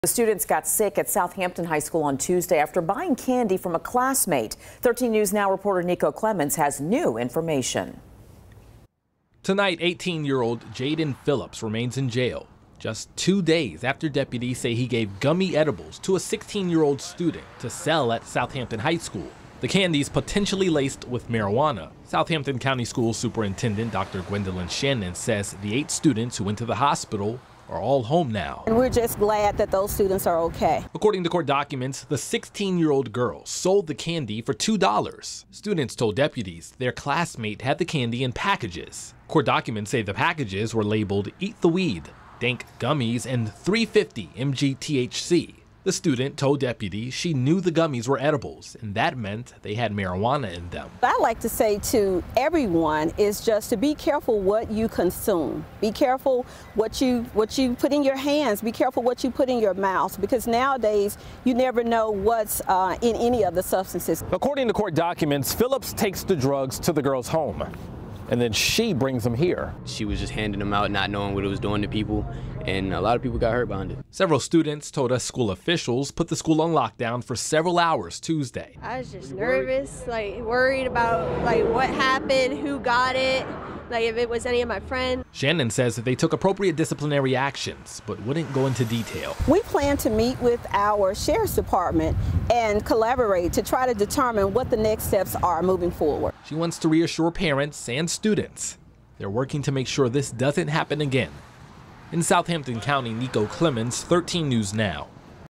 Court documents give more details about students hospitalized by candy
An 18-year-old is in jail after 8 Southampton High School students were hospitalized for eating laced candy.